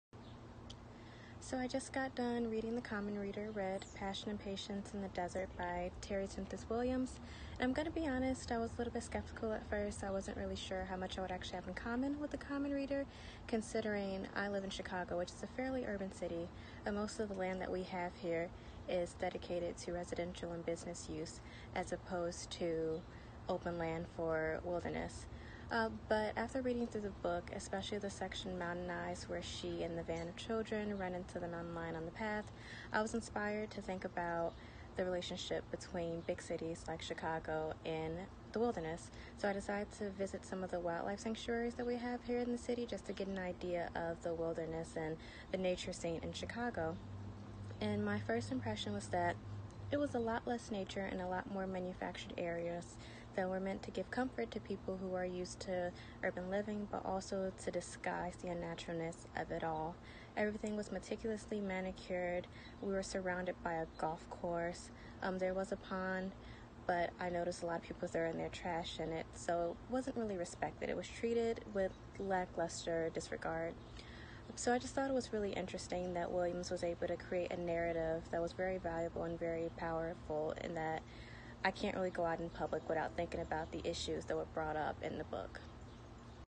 2020 Common Reader: Apryl Moore
Please join Sigma Tau Delta's Student Leadership as they discuss the 2020 Common Reader, Red: Passion and Patience in the Desert, by Terry Tempest Williams. This week’s video features Midwestern Region Student Representative Apryl Moore.